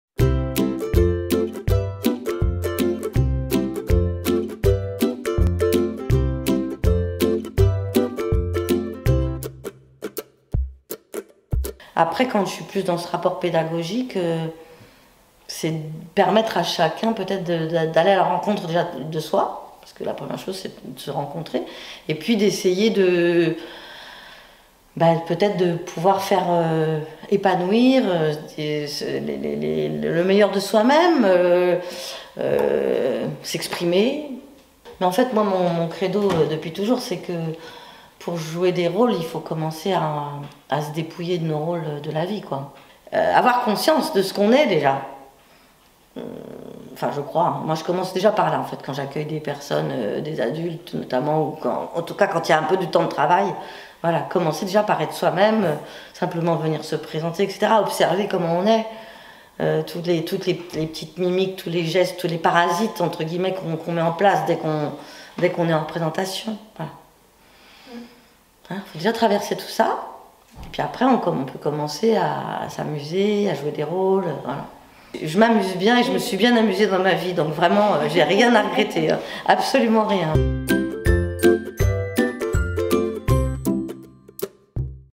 Portrait de Geneviève Delanné, comédienne
Cofondatrice de la compagnie de théâtre Les Têtes d'Atmosphère, Geneviève Delanné est une comédienne généreuse qui aime par-dessus tout la rencontre avec le public.
Réalisation : service Communication - Mairie de La Flèche / Juillet 2022